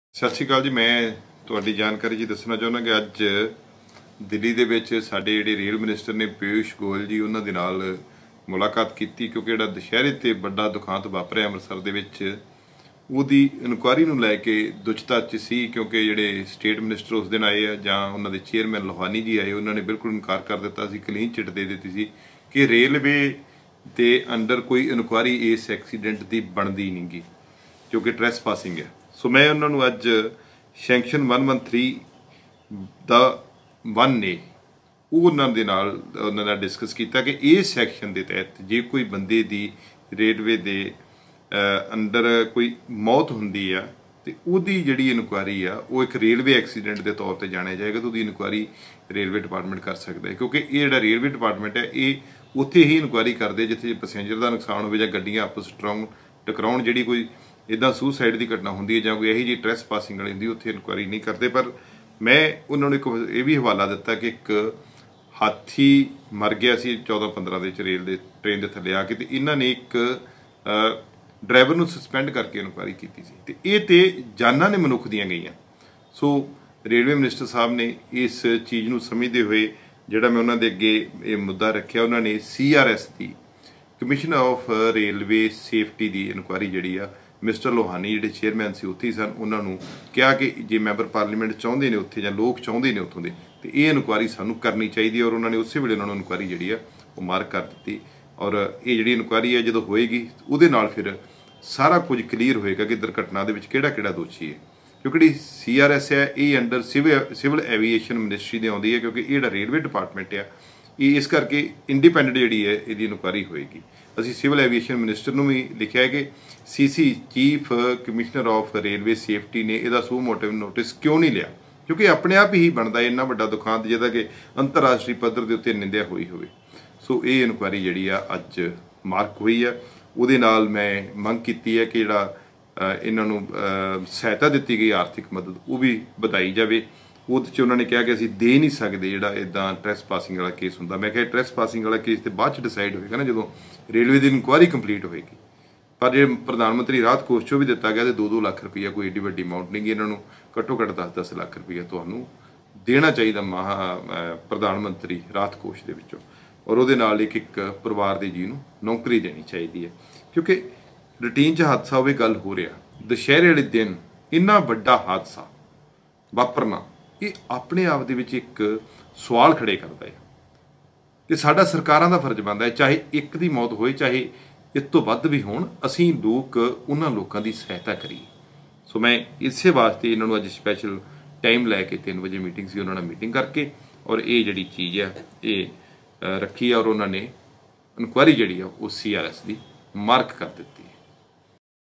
Amritsar Rail Hadhse De Vibhag Walo Jach Kite Jave Gayi-Gurjeet Singh Aujla-23-10-2018
AsiaTV Amritsaraccident Gurjeet Singh Aujla#Amritsar Police#Navjot Singh Sidhu#NavjotKaurSidhu